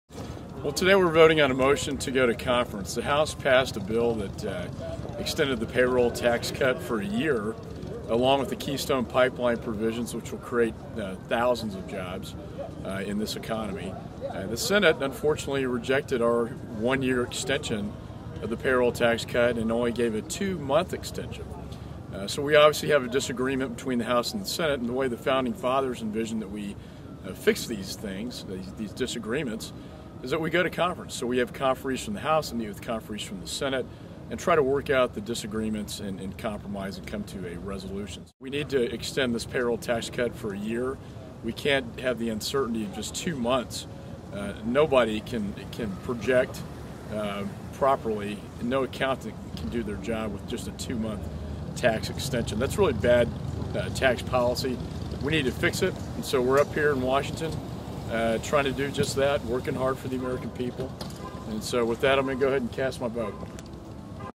Rep. McCaul on the Payroll Tax Cut
Congressman Michael McCaul discusses his vote to extend the payroll tax cut.